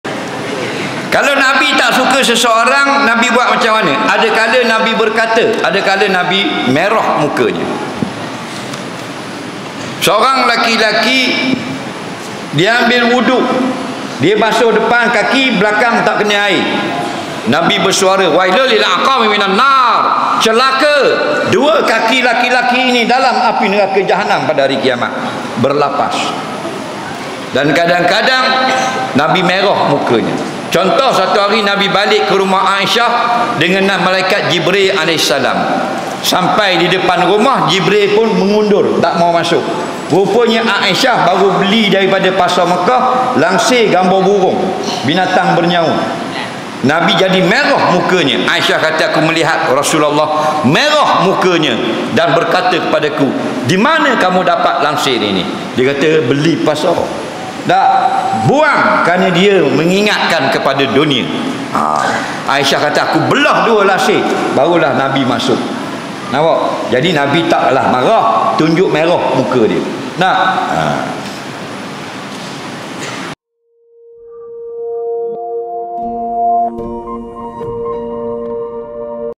Apa Nabi Buat Bila Tak Suka Pada Seseorang? - Ustaz Azhar Idrus
📆 30 September 2023 | Sabtu
⏰ 07.45 malam
🕌 Masjid Jamek Beserah, Kuantan Pahang
📖 Kitab :